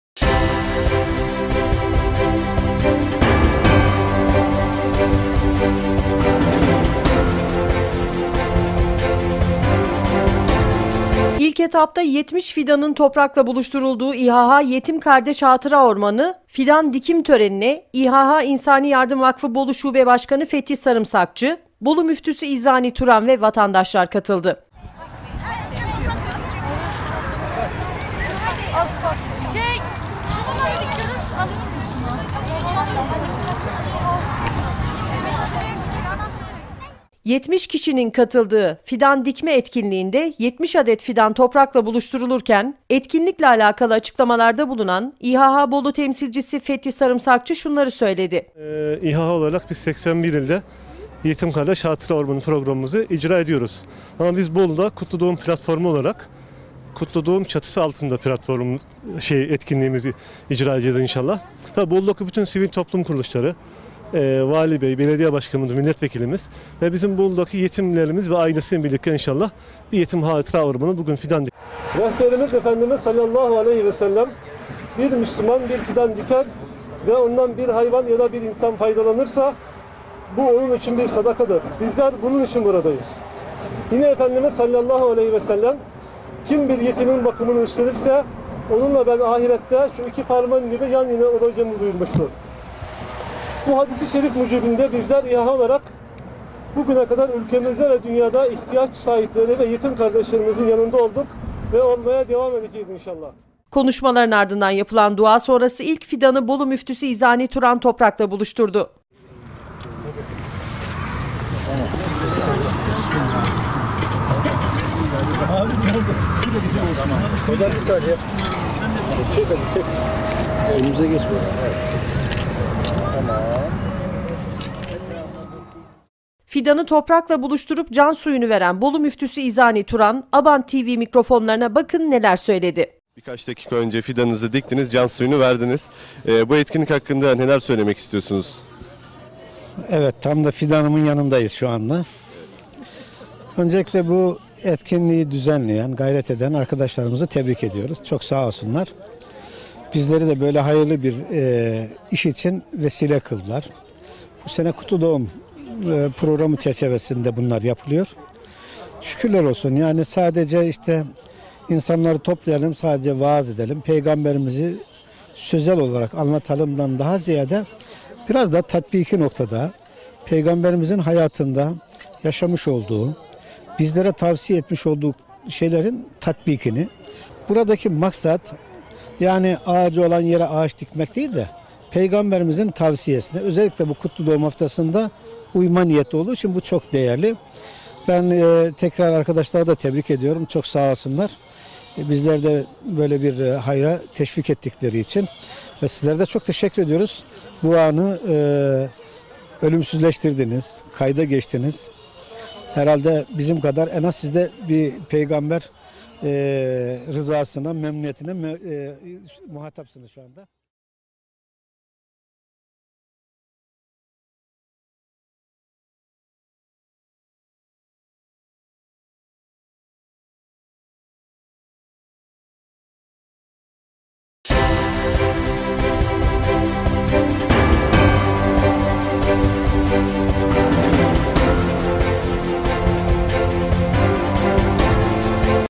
Bolu - İHH “YETİM GÜLERSE DÜNYA GÜLER”
İHH İnsani Yardım Vakfı Bolu Şubesi, Çaydurt Alıçören köyü mevkiinde ormanlık alanda Kutlu Doğum Haftası etkinlikleri çerçevesinde 70 fidan dikti. Fidan dikim törenine, İHH İnsani Yardım Vakfı Bolu Şube Başkanı Fethi Sarımsakçı, Bolu Müftüsü İzani Turan ve vatandaşlar katıldı.